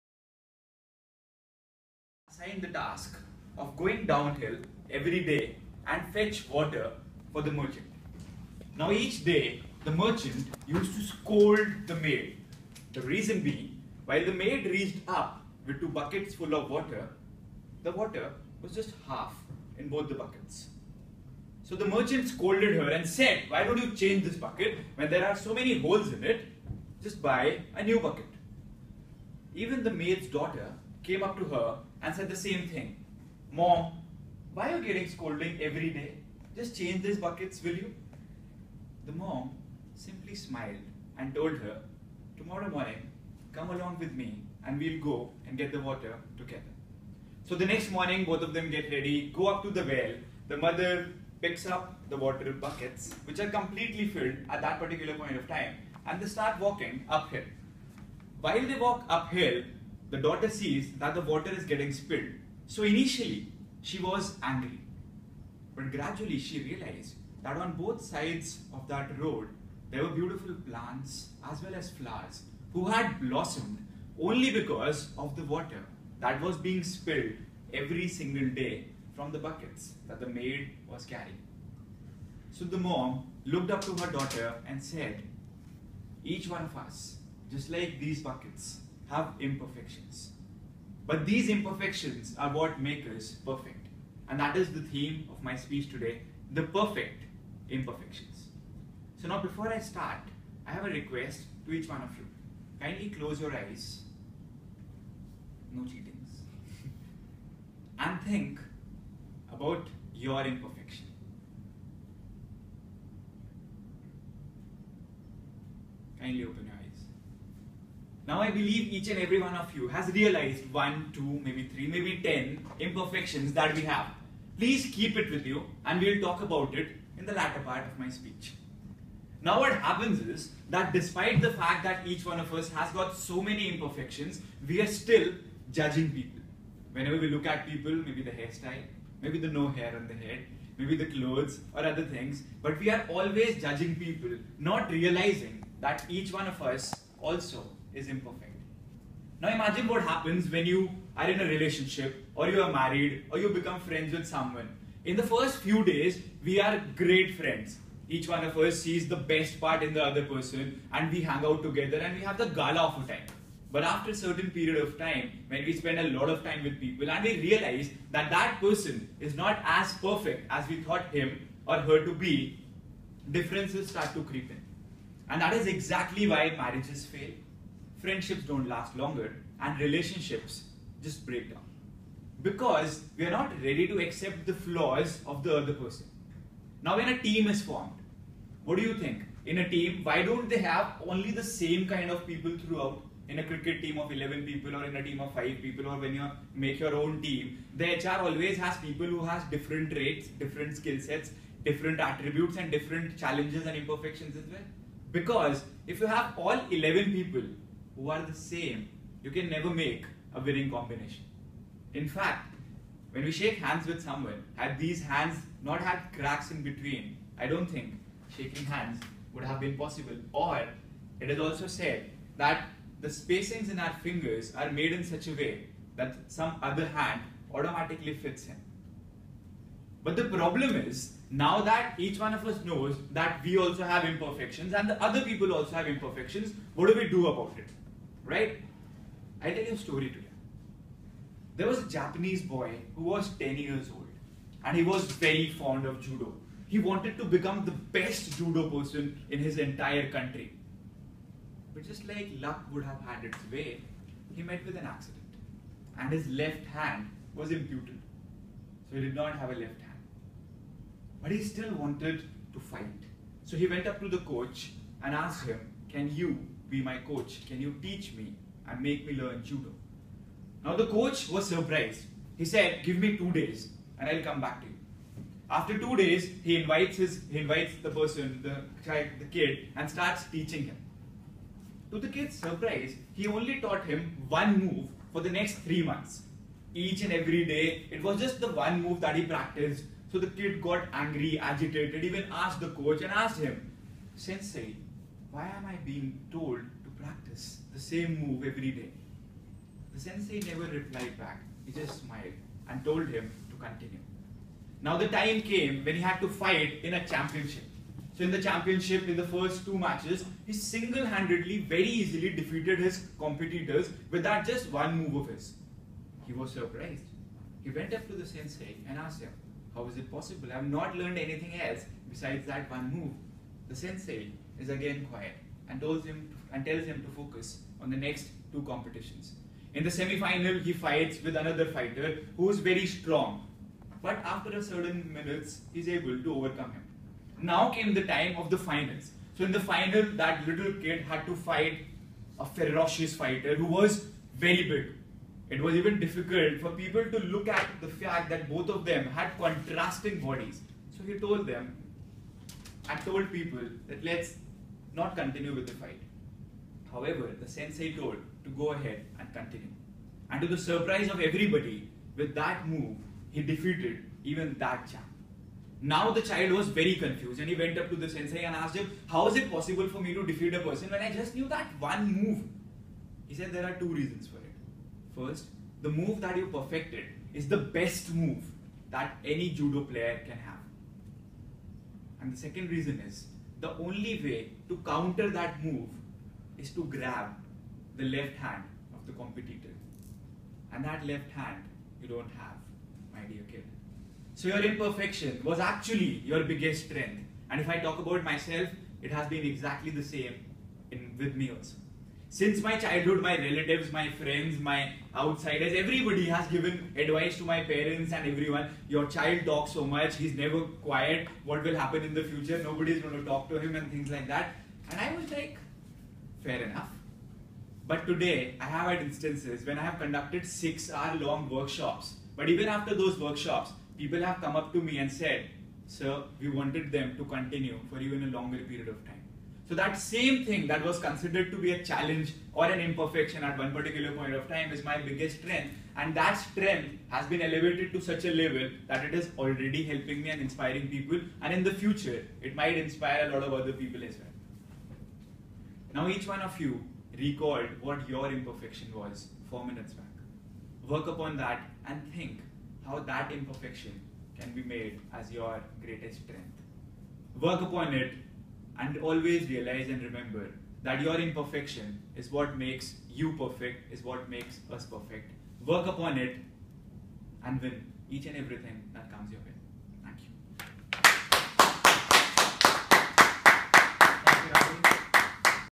46/50 - "The Perfect Imperfections"
The Forty Sixth Speech was delivered at Challengers Toastmasters Club, Gurgaon on 31st May.

Quite often we are dejected and depressed due to our short comings and imperfections.

Despite knowing the fact, that there doesn't exist any individual who is perfect, we still crib and complain about our faults.

This speech gives us various examples how our imperfections can actually be our greatest strength.